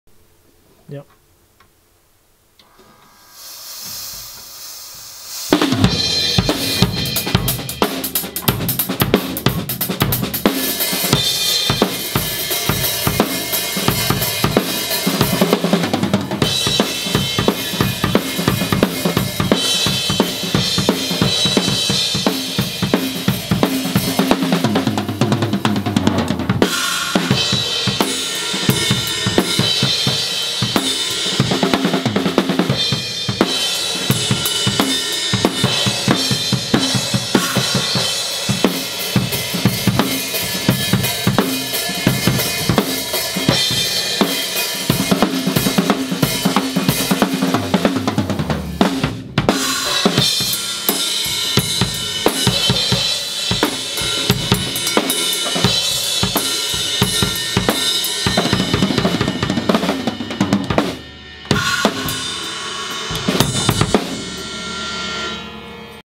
Adrian from Big Time drumming
Adrian Qvarfordt from Big Time playing drums after photosession.
Camera: Canon EOS 700D with rode videomic pro